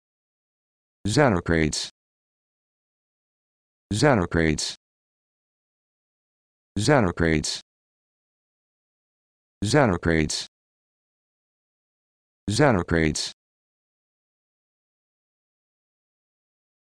How to Pronounce Xenocrates
This video shows you how to pronounce Xenocrates